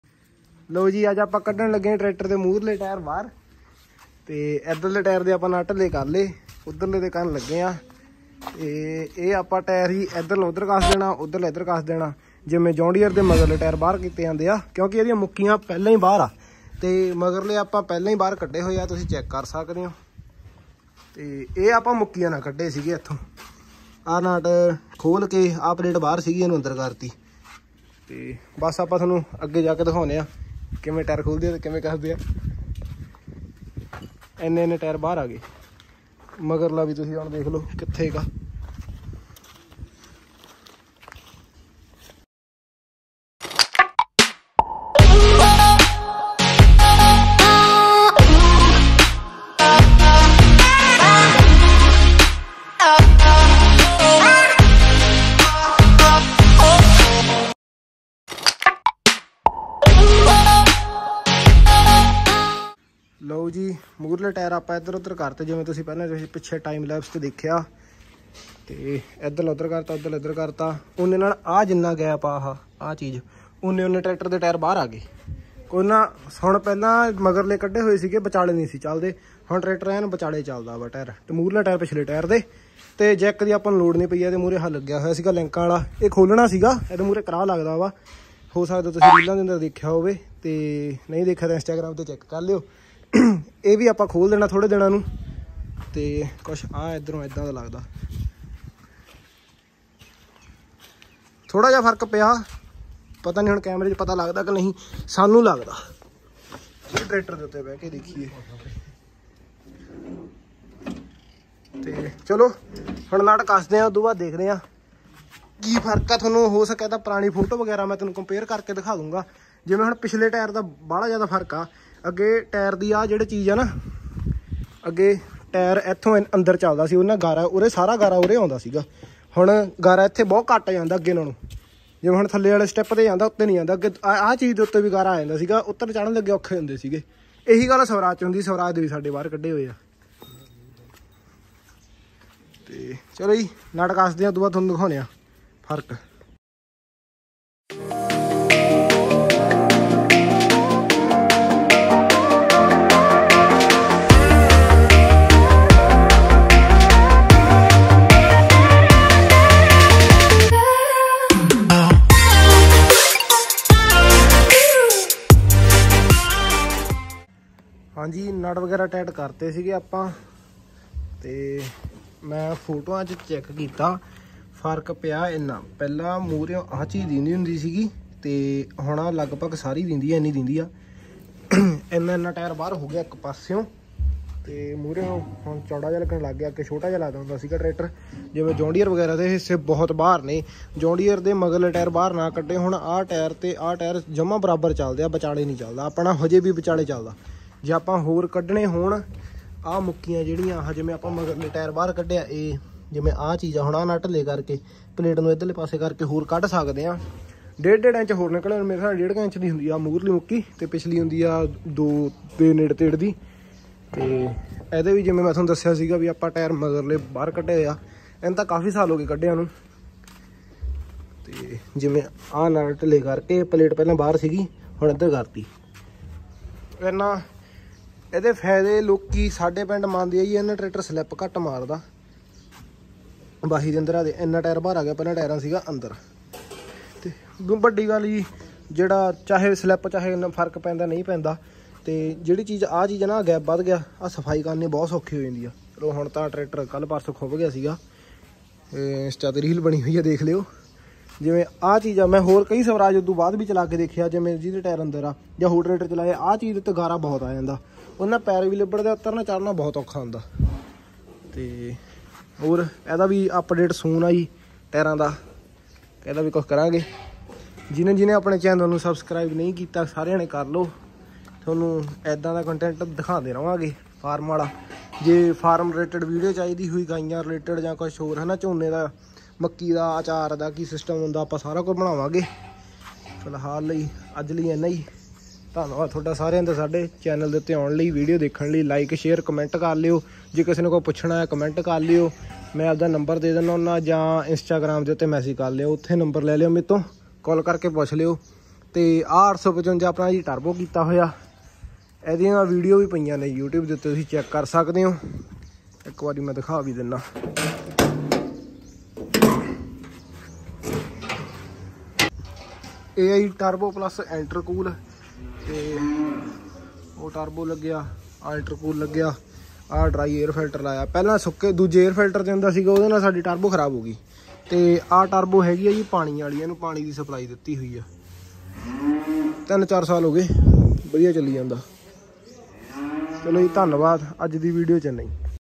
4X4 da 4 tyre kadd dita bahar / hun ne mar da slip / swaraj de turbo update soon 🔥
Tractir de tyre bhr kaddan da tarika te faida

How to fit turbo on Swaraj 855

Fordson 1952 model englania ford